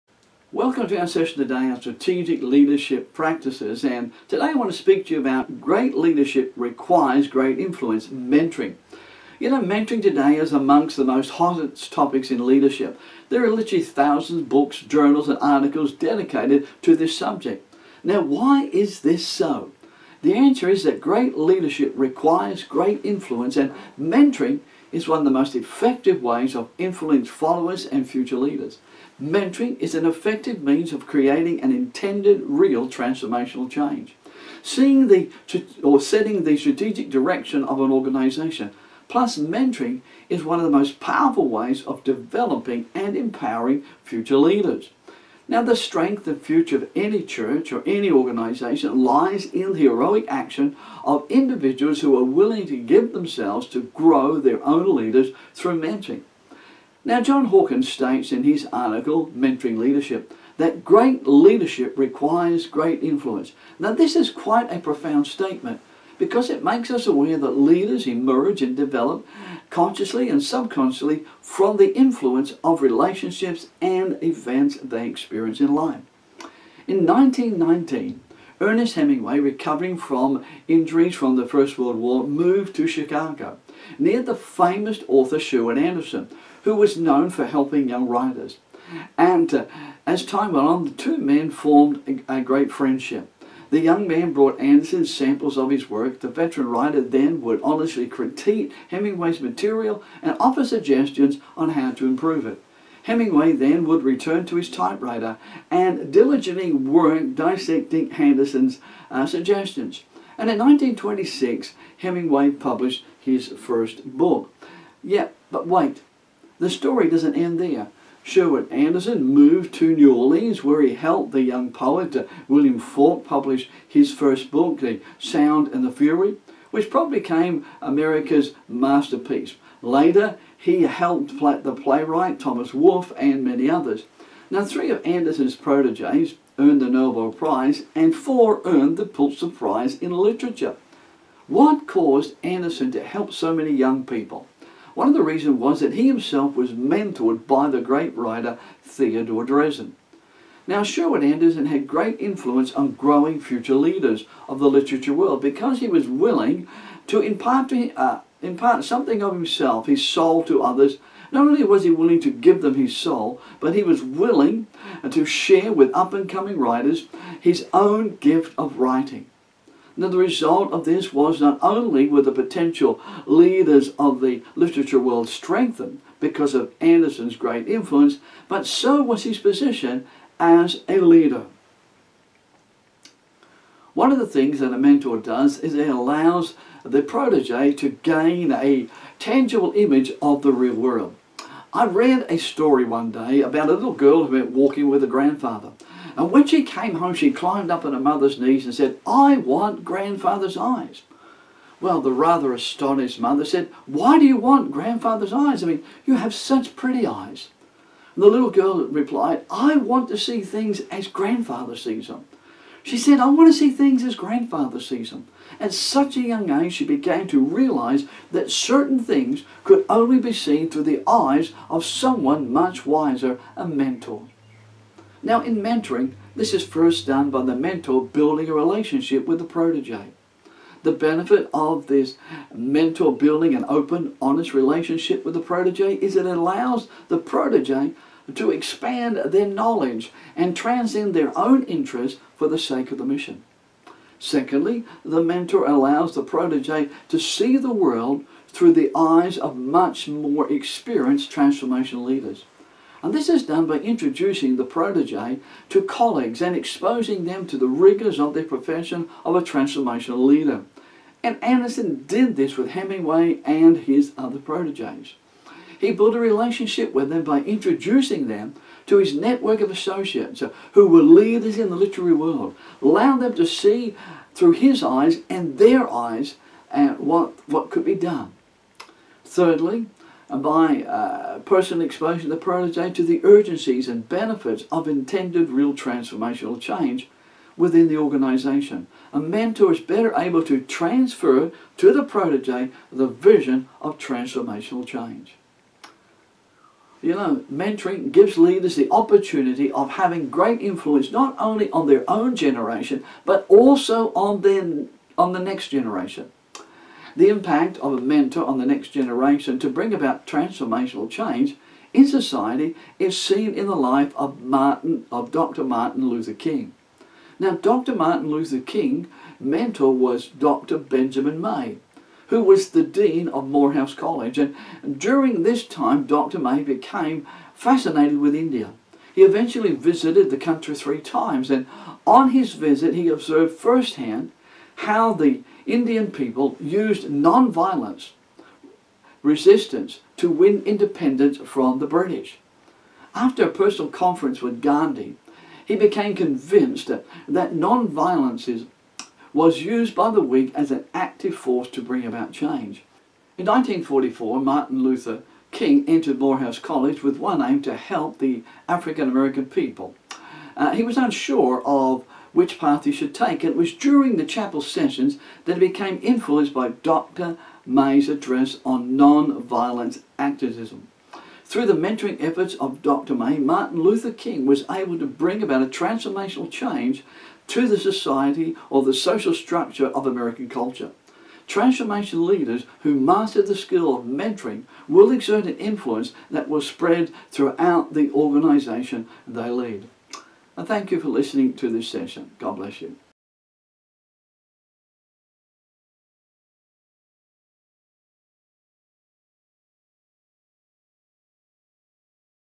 Strategic Leadership Principles N0:8 Great Leadership Requires Great Influence - Mentoring
Great Leadership Requires Great Influence - Mentoring
Produced by Trinity Leadership Products
Presented by Dr Tony Keys